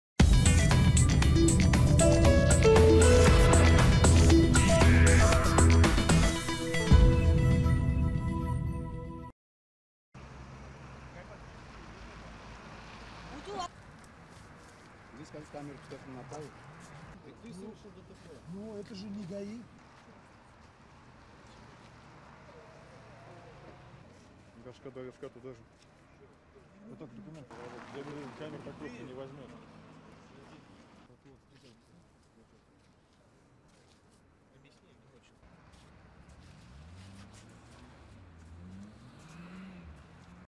В Казани Honda, уходя от полицейской погони, врезалась в забор
Несколько десятков человек стали очевидцами аварии на ул. Чуйкова. Там водитель автомобиля Honda на большой скорости врезался в забор. Оказалось, что ДТП произошло после полицейской погони.